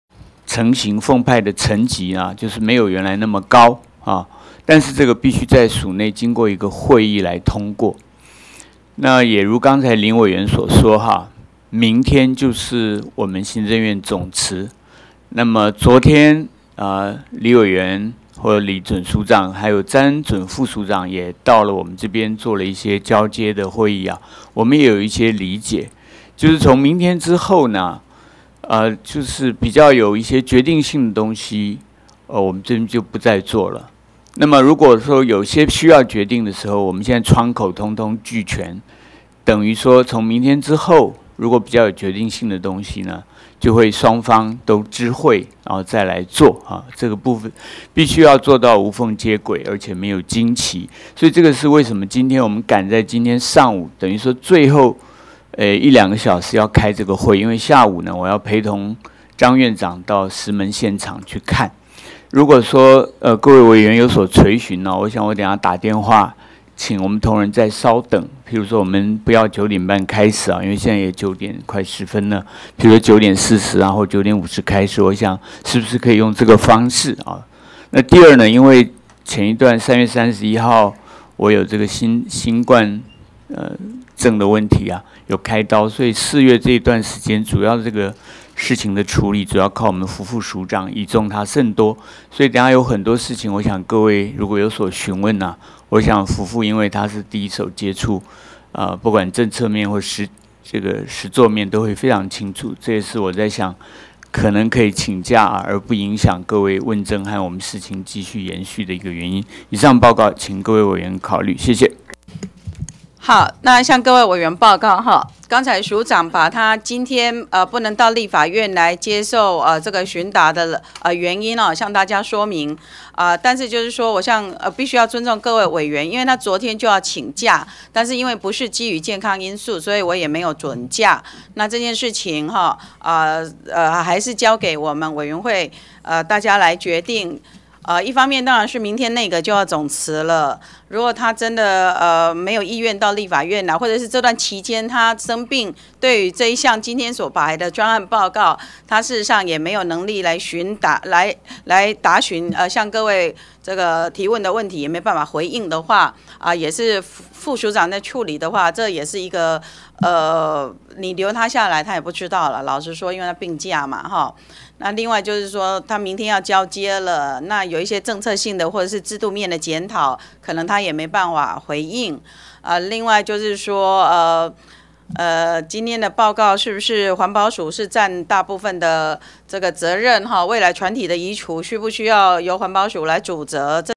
20160511 環保署長 魏國彥 說明 為何今日想請假 林淑芬 油汙 德翔台北 社福衛環 【立法院演哪齣？】
y2016 m05 d11 custom hd c12